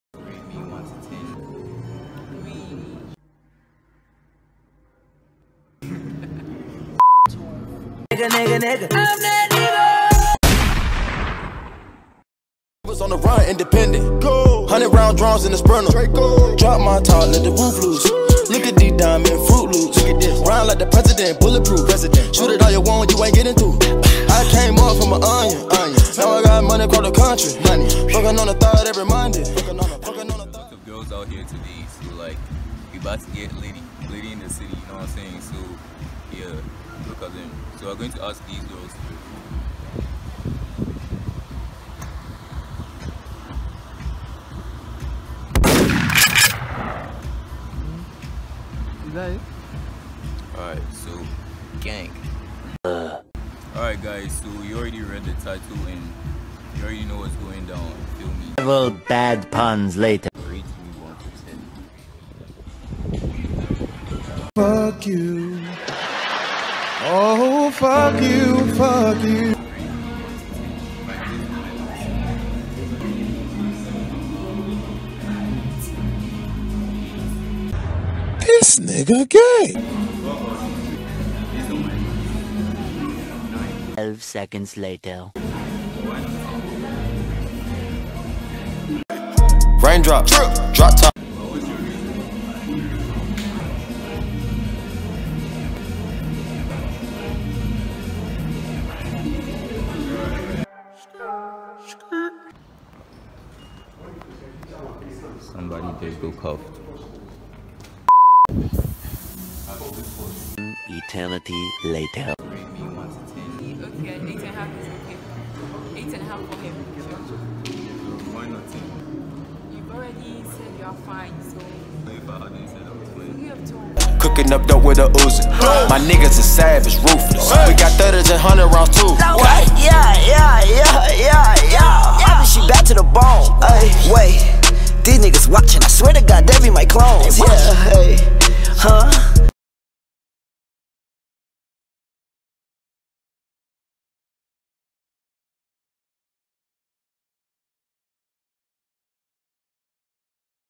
ASKING GHANAIAN GIRLS TO RATE ME 1-10 (HER BOYFRIEND GOT MAD) 😳😋
ASKING GIRLS TO RATE ME 1-10 PUBLIC INTERVIEW.

Let me know what other videos you’ll love to see on my channel!